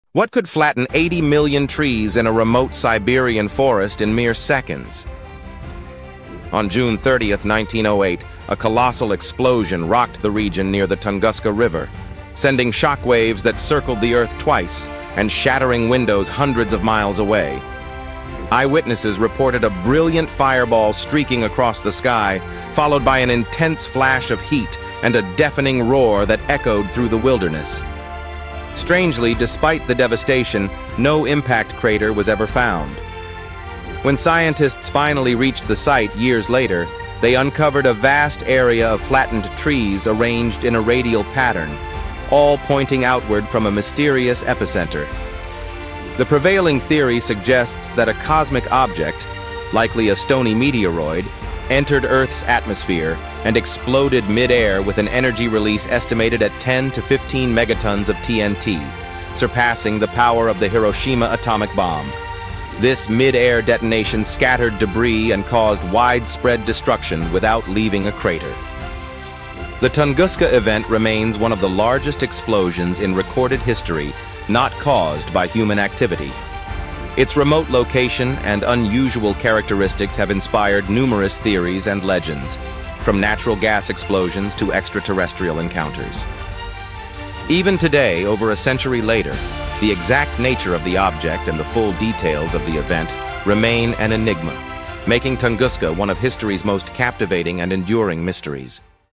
The Tunguska Mystery: A Forest Flattened Overnight
A blast more powerful than an atomic bomb leveled a Siberian forest in 1908. What really happened in Tunguska?